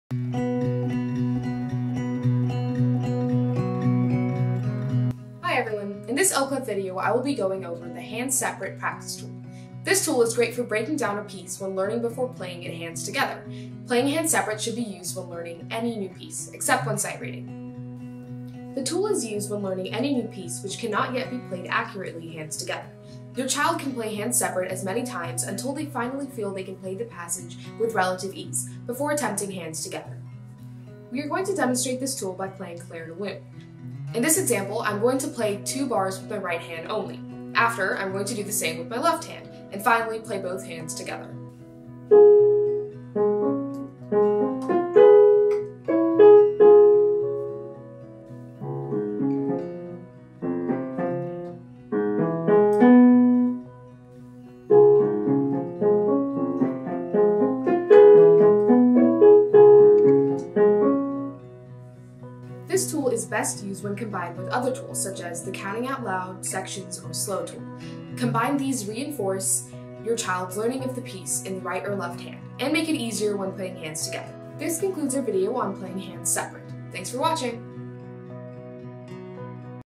Hands Separate | Oclef Method: Tools
The Oclef Method has a collection of tools that piano students us to help them diagnose, understand and solve problems in their learning. This series of videos briefly explains the purpose of each of the 25 tools and how to use them to improve problem solving and learning.